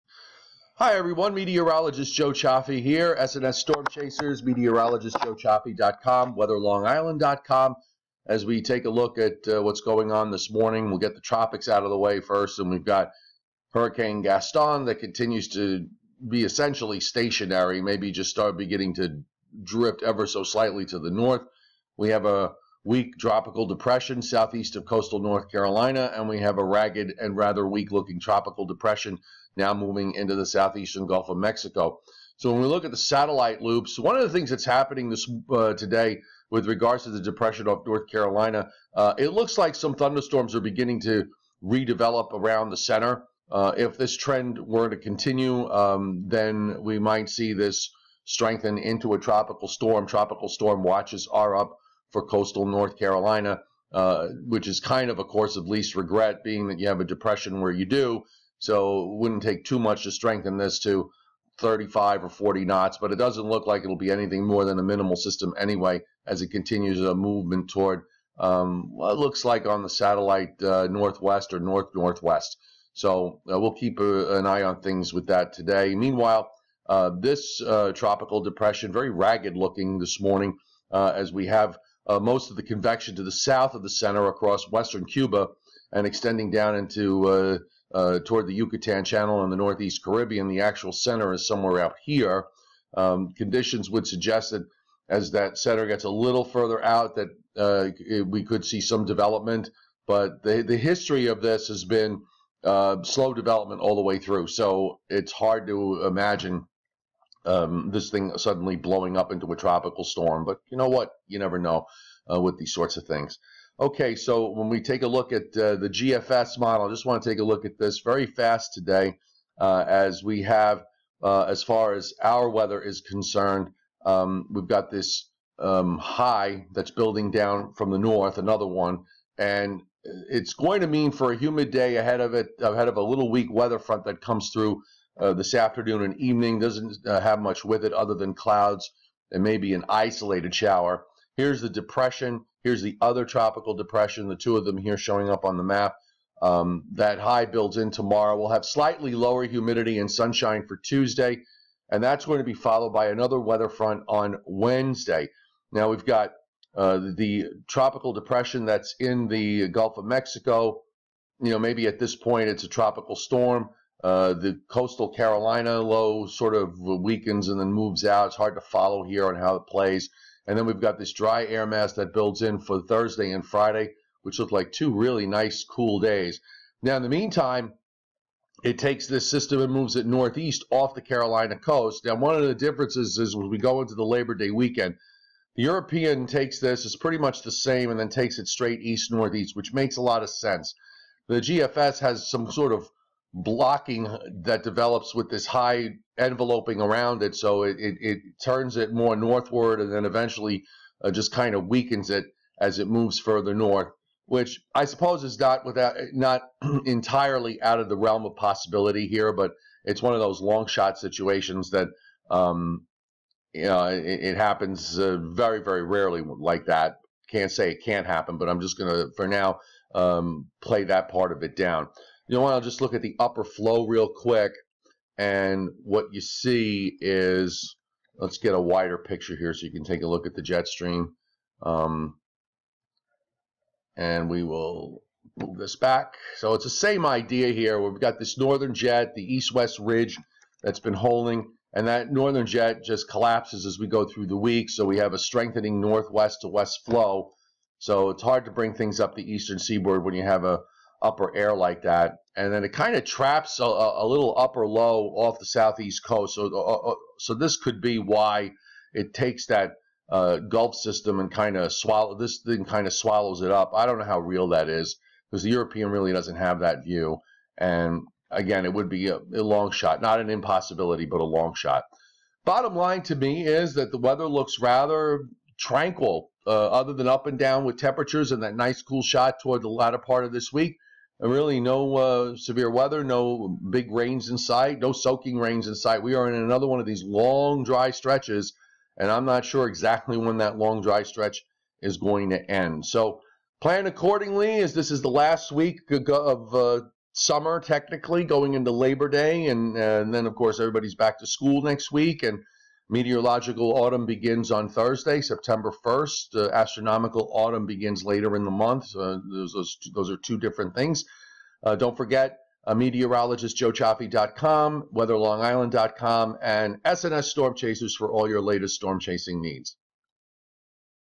Hurricane Gaston, Tropical Depression 8, Tropical Depression 9 08292016
Hurricane Gaston, Tropical Depression 8, Tropical Depression 9 continue in the Atlantic. Long Range GFS model forecast through Labor Day.